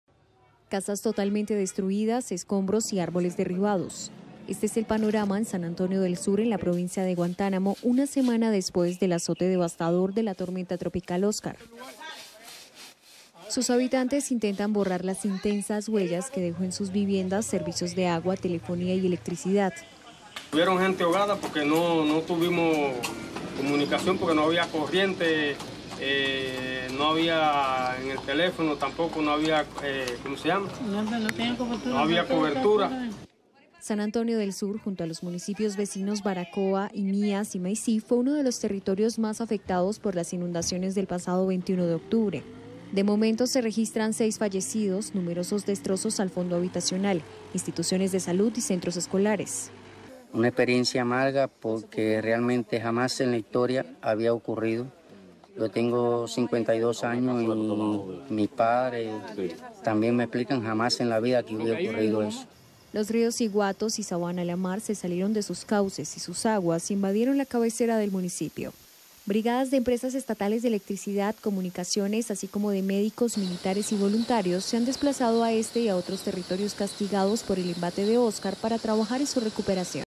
Cuba intenta recuperarse tras el paso del devastador huracán Oscar
👉 Su paso por la isla coincidió con una tensa situación, luego de que todo el país quedara a oscuras por varios días debido a una desconexión y colapso del sistema energético nacional.